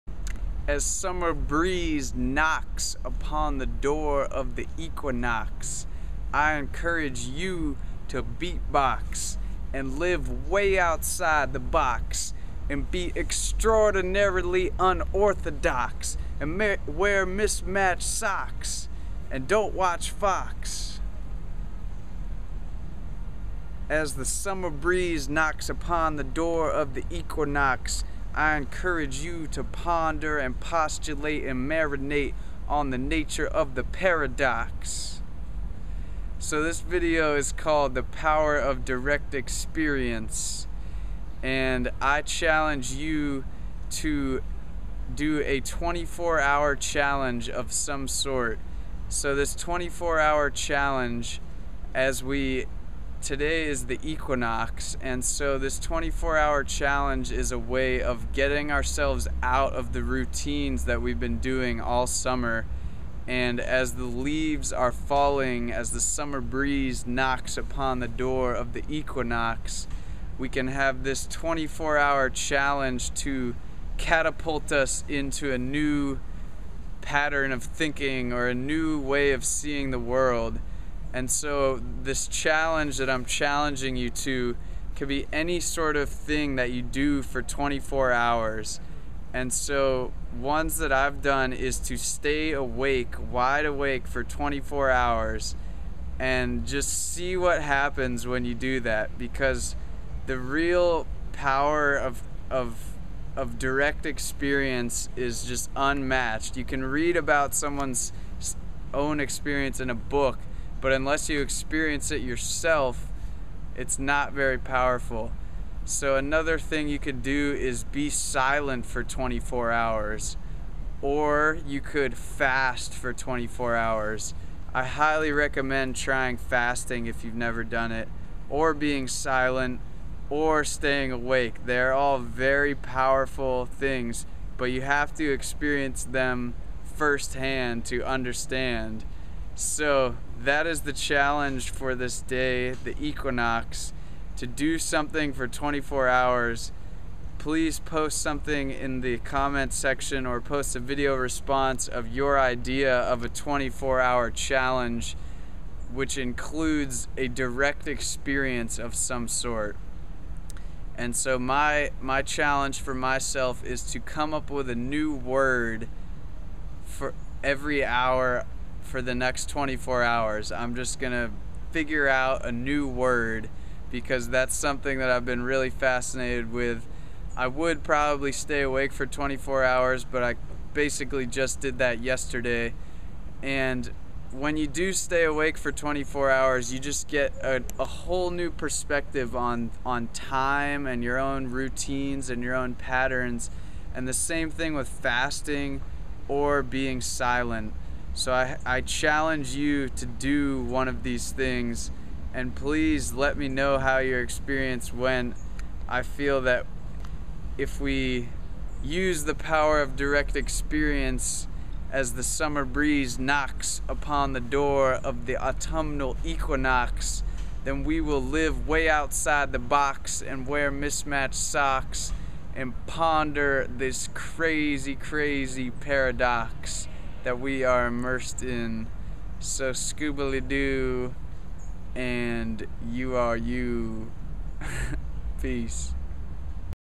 The Power Of Direct Experience - Phil Osophical
As the summer breeze knocks upon the door of the Equinox, how will you live outside THE BOX? I challenge you to do something wild for a full 24 hours...Stay awake for 24 hours!...Only consume water for 24 hours!...Don't speak a word for 24 hours...Choose your own challenge, the only important thing is DO IT!!! The power of our own direct experience is extremely transformative and allows us to reflect on our normal routines. I also recommend writing in a journal before, during, and after your 24 hour challenge.

Please share any ideas for a 24 hour challenge in the comments, by video response, or on Phil Osophical's blog @ Facebook.com/Philsideas